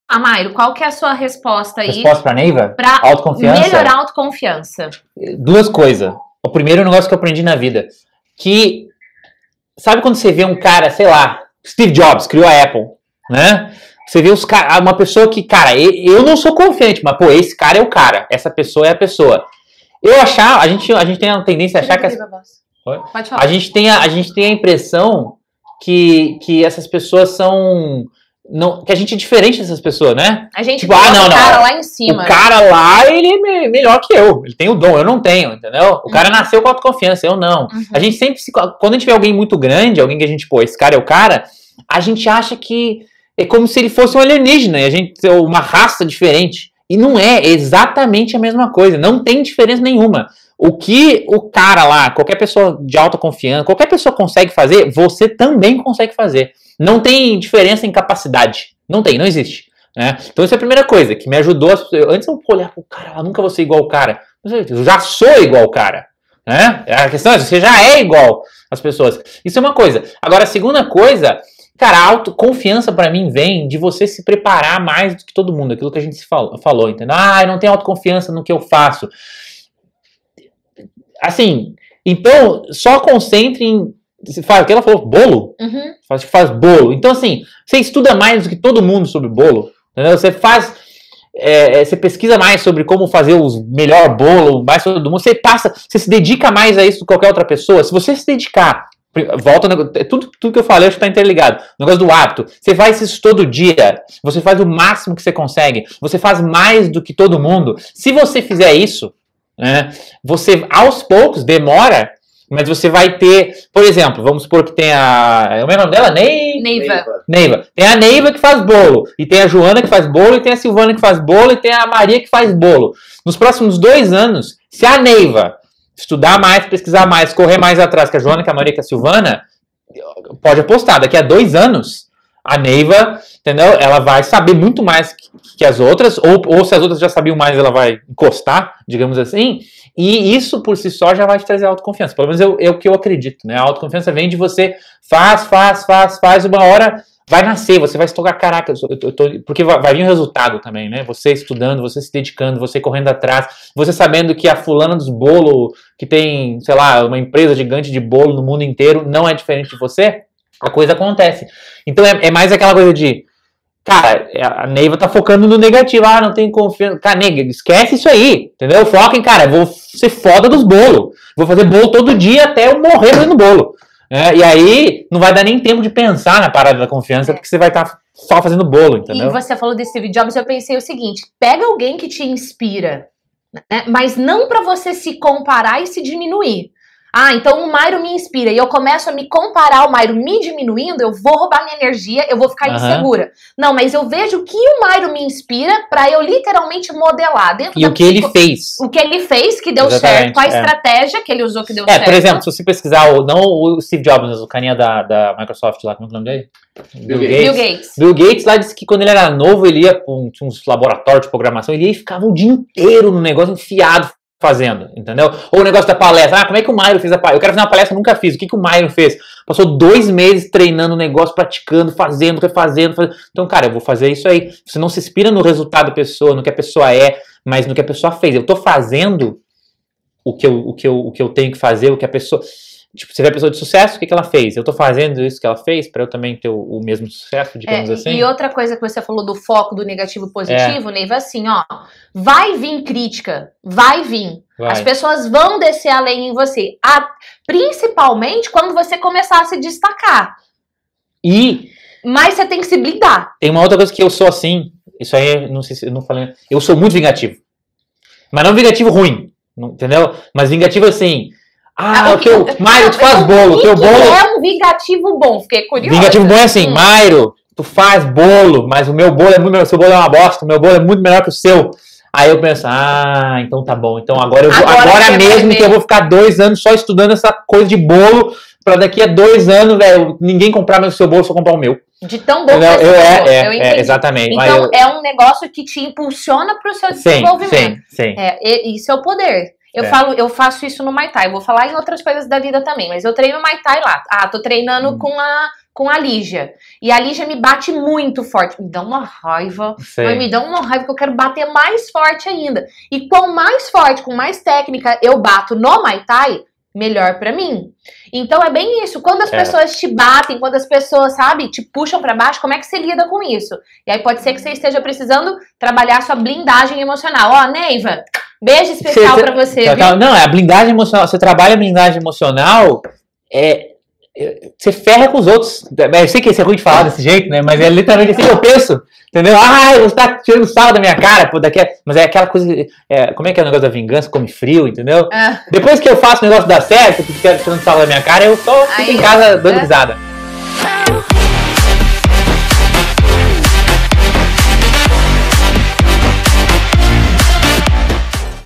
Como ter autoconfiança?
Como ter autoconfiança? Você já se perguntou como ter autoconfiança pra alcançar suas metas? Como ser seguro de si? O que você pode fazer para aumentar a sua autoconfiança? O que faz você não se sentir auto-confiante? A autoconfiança é a sensação que uma pessoa possui em si mesma de que ela é capaz de fazer algo. No clipe de hoje, eu, Gislene Isquierdo, e meu convidado especial Mairo Vergara falamos um pouco sobre como ser autoconfiante.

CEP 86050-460